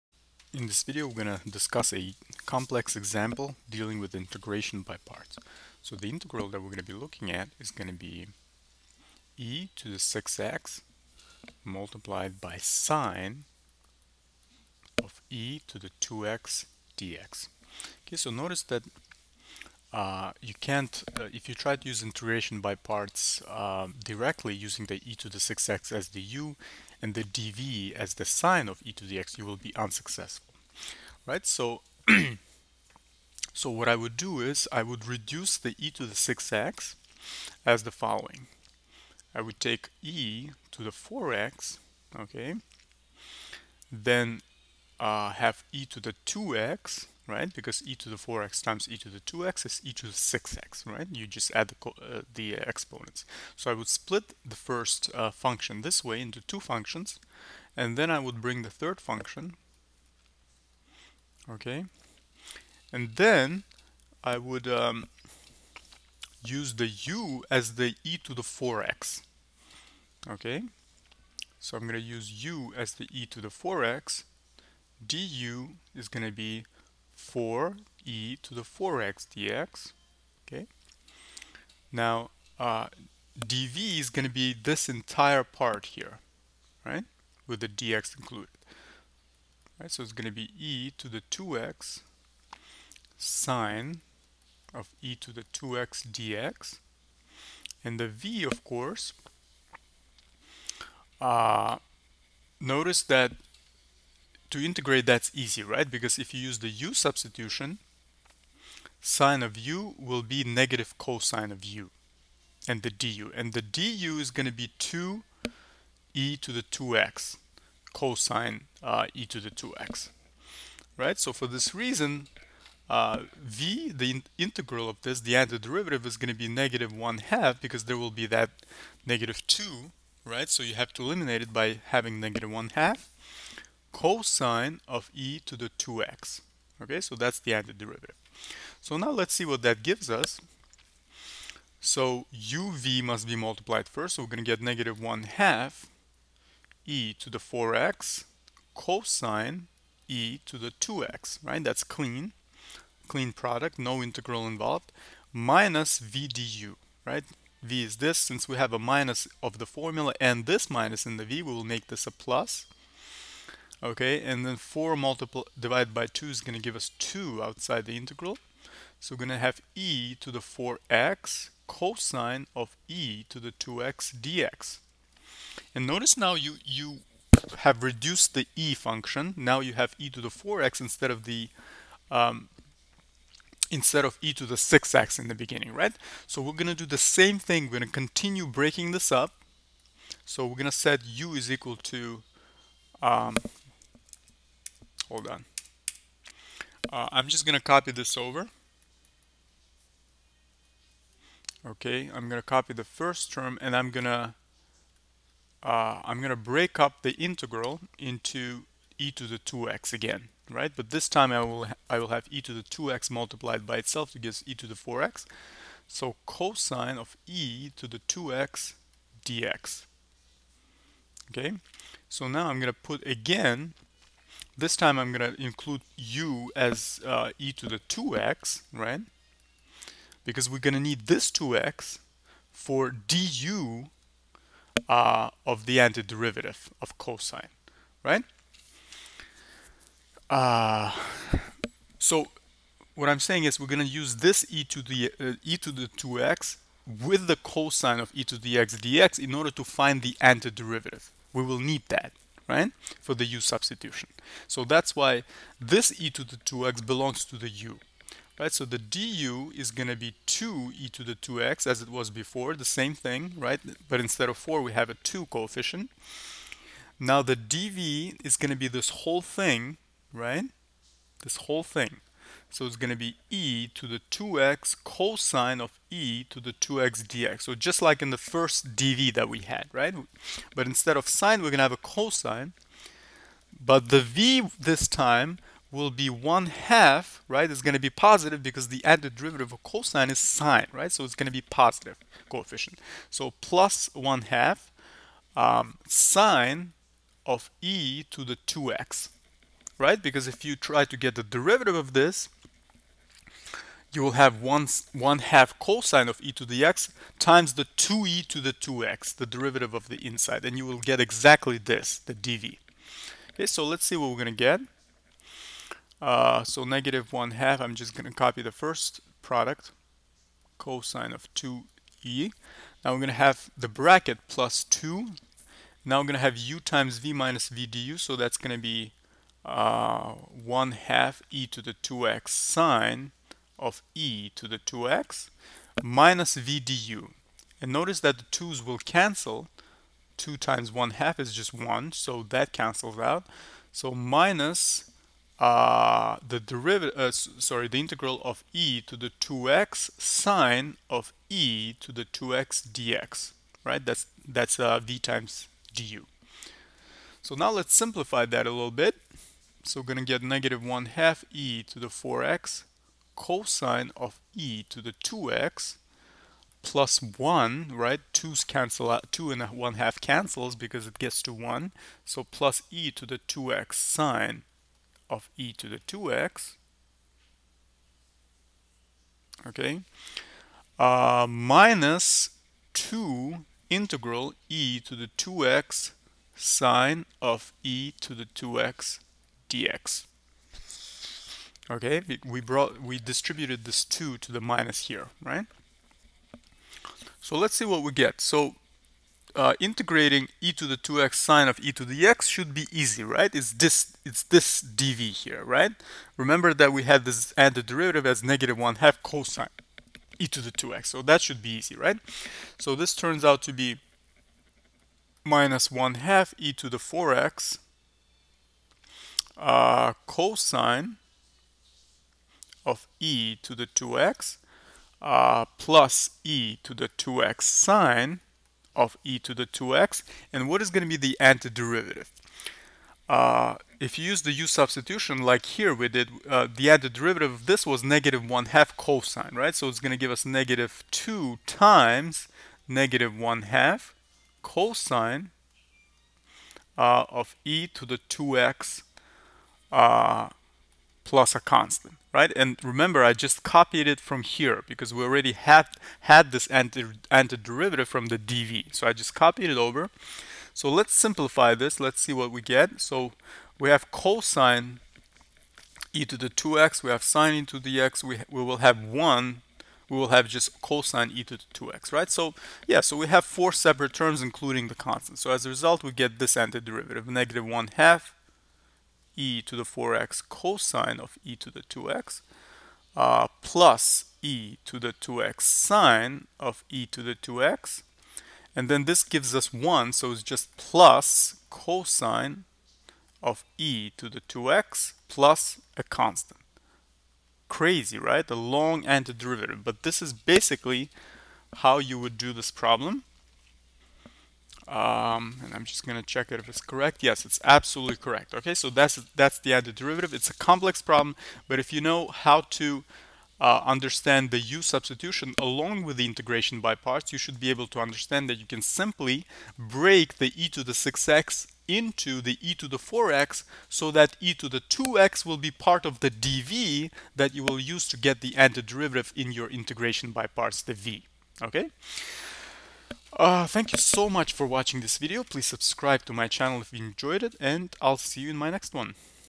🖇 Extremely HARD Integration by Parts problem ! ! ! ! !
In this video you will learn how to find the Antiderivative of a function f(x) = e^(6x)sin(e^(2x)) by using Integration by Parts method.

0:17 Rewriting the integrand before applying Integration by Parts
1:05 Choosing u and dv
2:55 Repeating the same step - rewriting the integrand
3:42 Choosing u and dv
5:15 Solving and simplifying the Integral

Definite Integral:
f(x)=lnx, [1,10]

Definite Integral:
f(x)=xsin2x, [0,1]

Antiderivative:
f(x)=cosxcos2x

Antiderivative:
f(x)=x^2lnx

Antiderivative:
f(x)=xcosx